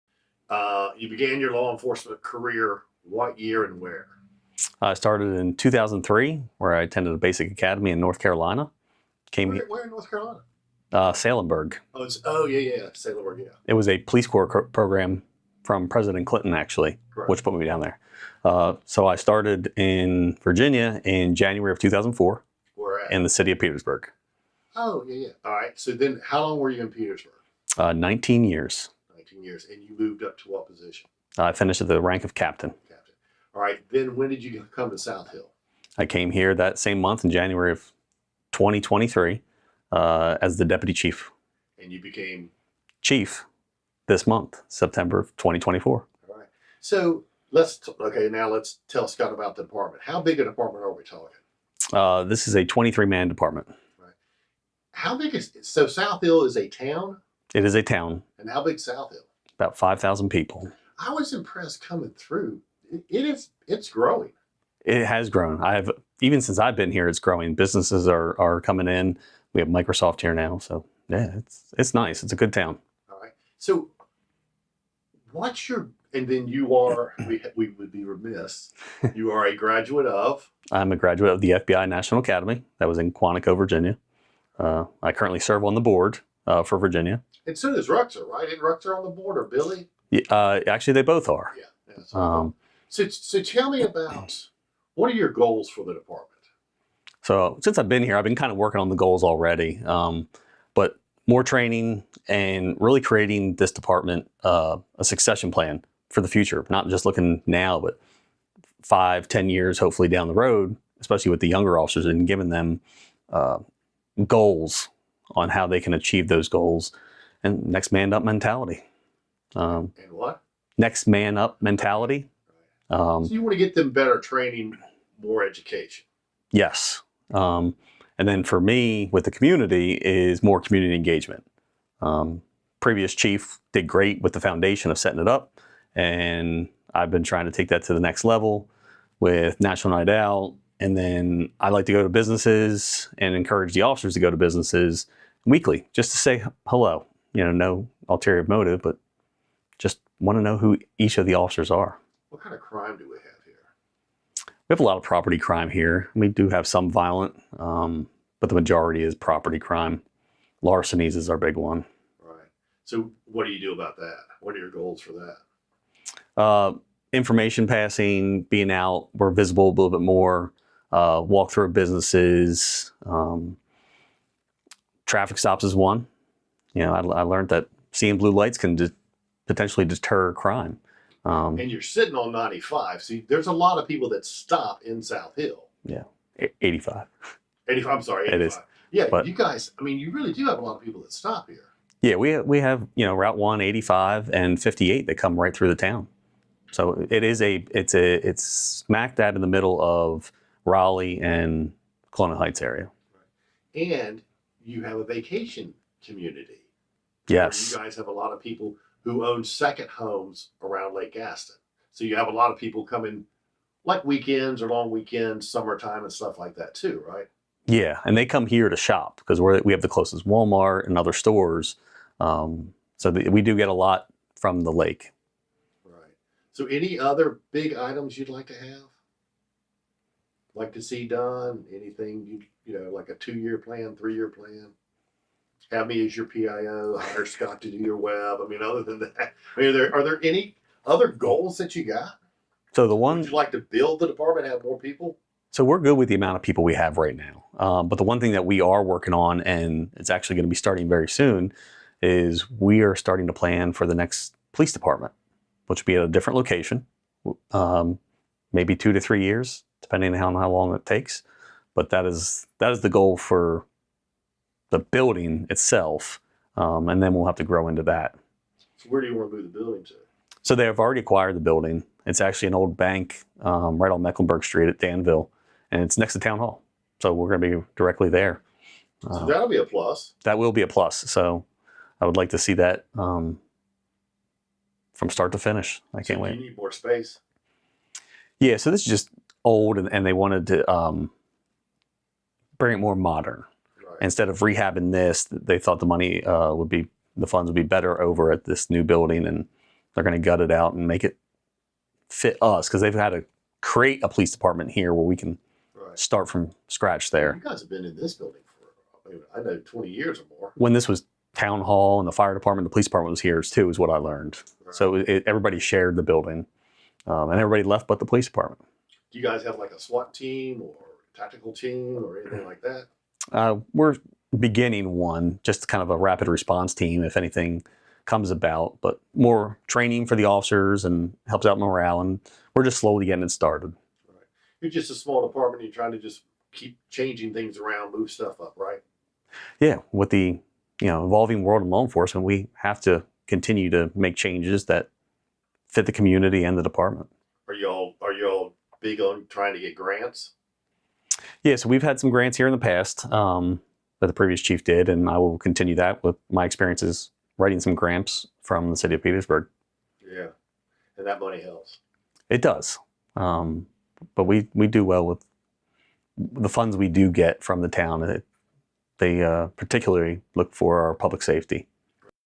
Meet the new South Hill Police Chief Greg Geist
New South Hill Police Chief Greg Geist is no stranger to the town which is located about 80 miles southwest of Richmond off Interstate 85 and Route 58.